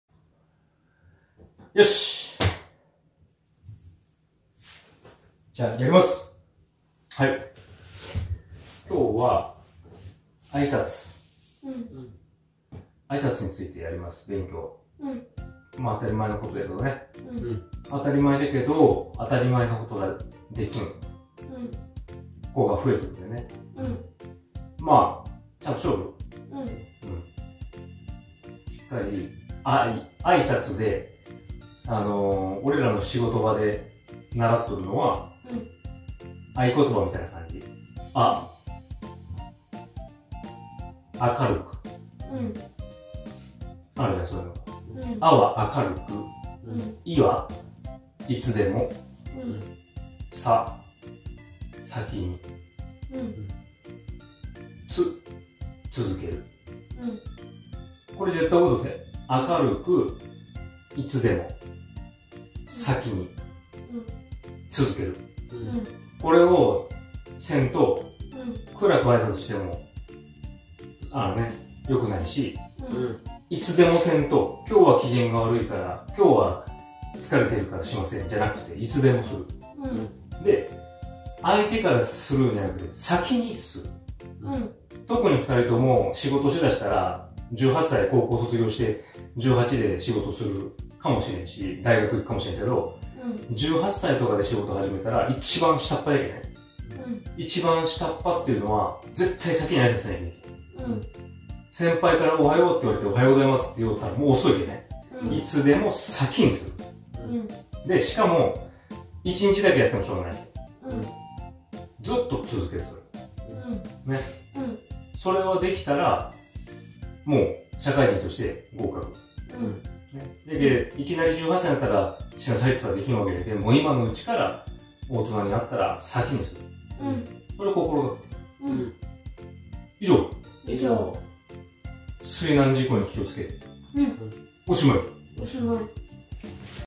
学校では習わない！【挨拶】について勉強しました！
学校では習わないけど、社会に出ると必ず役立つ勉強をやります！
少しは習うけど、先生からはあまり言わなくなったような気がします。
兎に角、挨拶をしておけば仕事はうまく回ります！